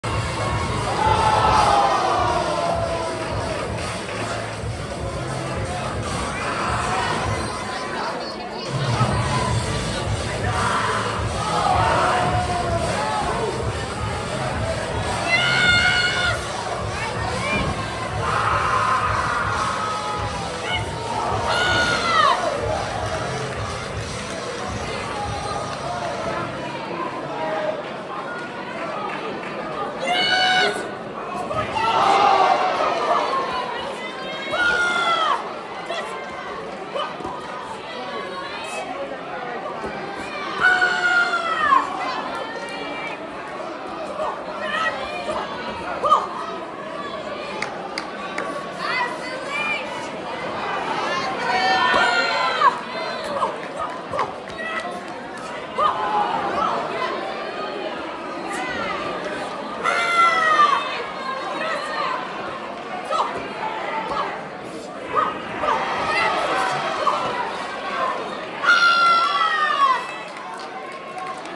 Lilli McGuire Women's CMX Forms Runoff - Diamond Nationals 2024
Lilli McGuire of KICK Team competing in the women's CMX forms runoff at the 2024 Diamond Nationals in Minneapolis, Minnesota.

Special thanks to Mike Brumme for his contributions to documenting the events at the Diamond Nationals.